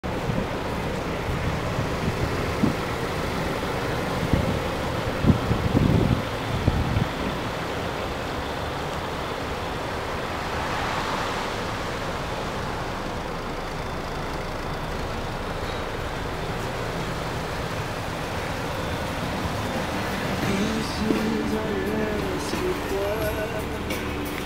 2014 Jeep Cherokee Sport | True Blue | EW226904 | Seattle | Bellevue
Five star rated!! -- Cherokee Sport, 4D Sport Utility, 2.4L I4 MultiAir, 9-Speed Automatic, 4WD, Blue Pearl, Black/Iceland Gray w/Cloth Low-Back Bucket Seats, and **BLUETOOTH**.